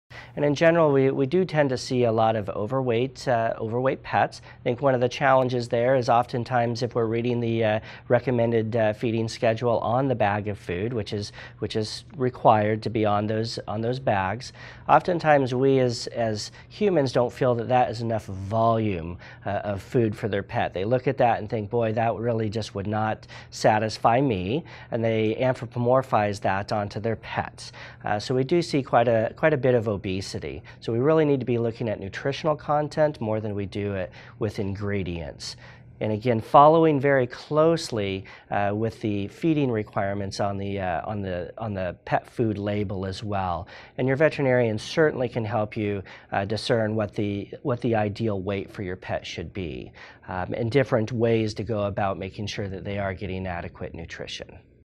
Dog (and Cat) Weight Management
And in general, we do tend to see a lot of overweight pets. I think one of the challenges there is often times if we are reading the recommended feeding schedule on the bag of food, which is required to be on those bags.  Often times, we as humans don’t feel that that is enough volume of food for their pet. They look at that and think boy that really just would not satisfy me and they anthropomorphize that on to their pets. So, we do see quite a bit of obesity so we really need to be looking at nutritional content more than we do with ingredients and again, following very closely with the feeding requirements on the pet food label. As well and your veterinarian certainly can help you discern what the ideal weight for your pet should be and different ways to go about making sure that they are getting adequate nutrition. .

1)This video has no negative impact.
2)This video is also for teaching & Informational AND Entertainment purposes.
3)It is not transformative in nature.